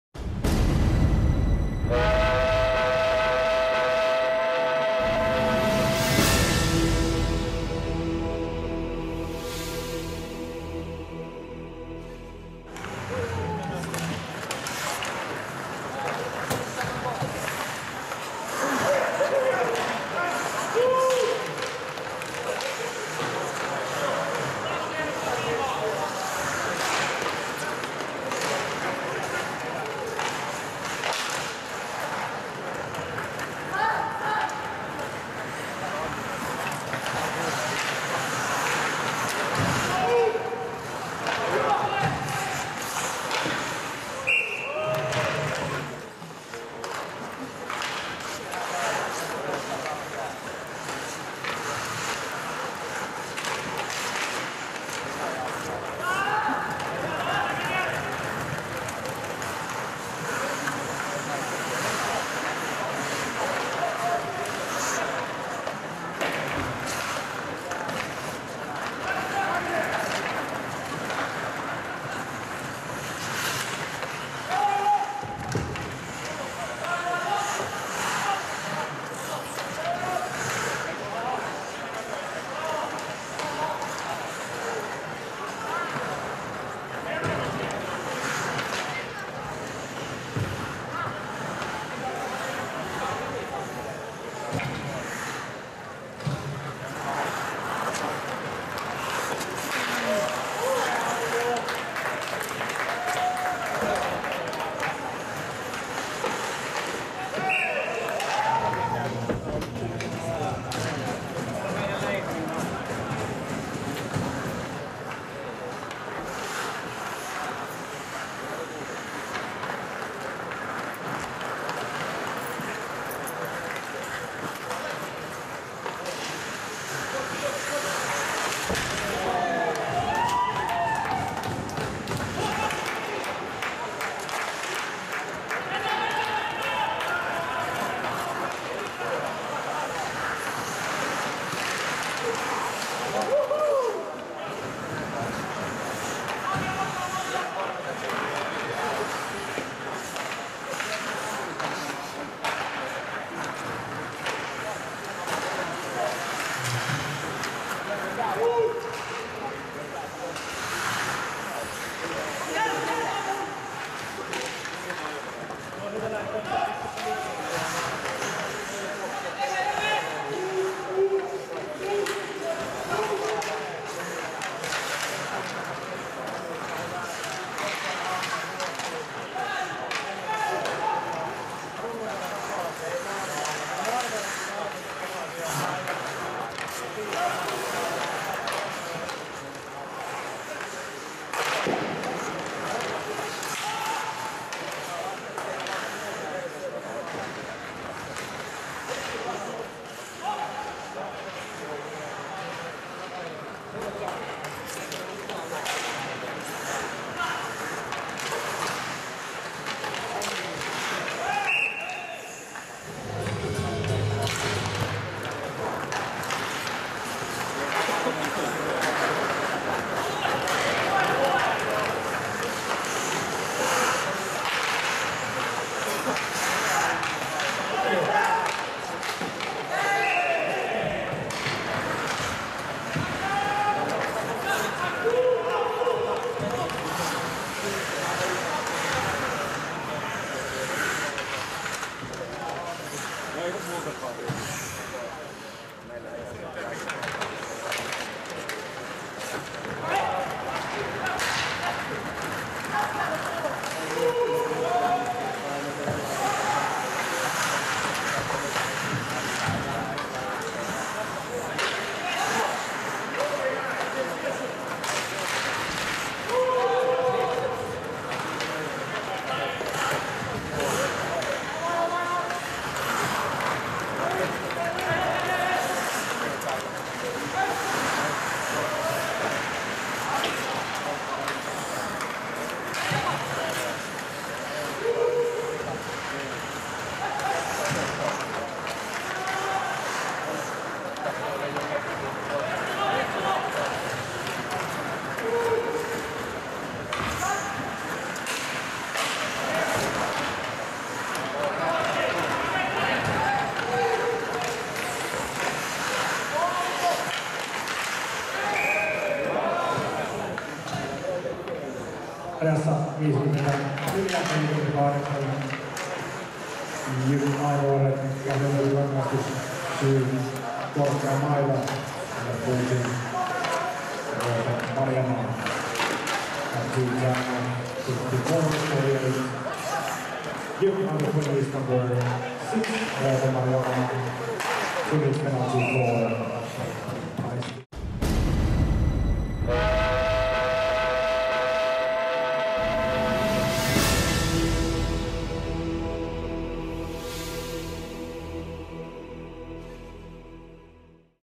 Межсезонье-2013: JYP Jr.A - "Локо" - 0:6
В городе-побратиме Ярославля, Ювяскюля, молодежный состав "железнодорожного" клуба был встречен также радушно, как и в других населенных пунктах Финляндии. Правда вот местная команда вряд ли оказалась этому рада. Парни Петра Воробьева учинили противнику очередной разгром.